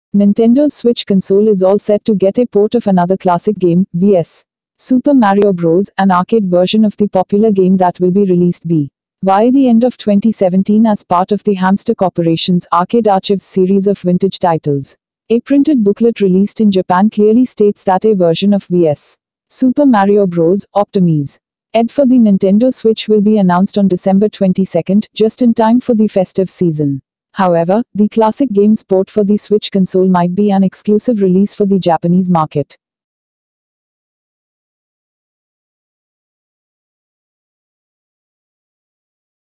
The Original Super Mario Bros. Arcade Game Is Coming To Nintendo Switch
Earlier this year, the developers at Hamster expanded its Arcade Archives line-up for the Nintendo Switch by introducing an old-school port of the two-player Mario Bros., alongside its many Neo-Geo releases that have been coming out for the system at that time.